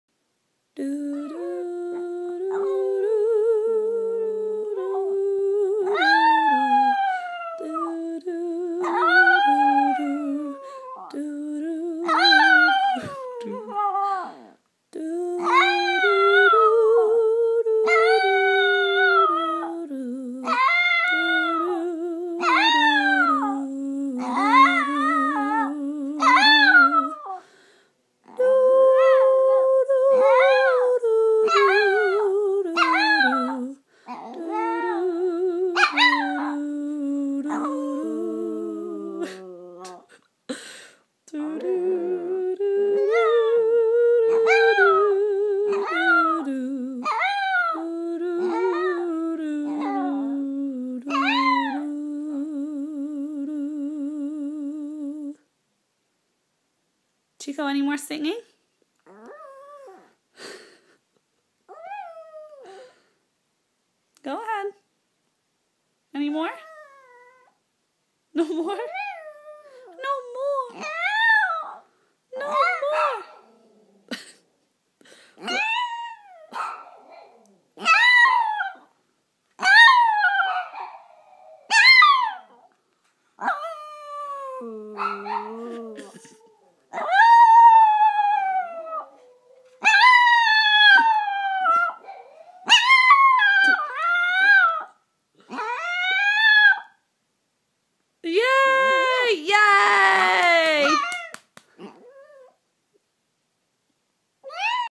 Chico the Chihuahua singing Christmas Carols
Whenever I sing or music is on in the house, whether it be loud or soft, piano or recorded music, high or low, my 5 year old chihuahua, Chico Suave, always sings along, prompting my yorkie, Lola, to do the same! Thought it hurt his ears at first but then realized it wasn't, I just have a musical pup :)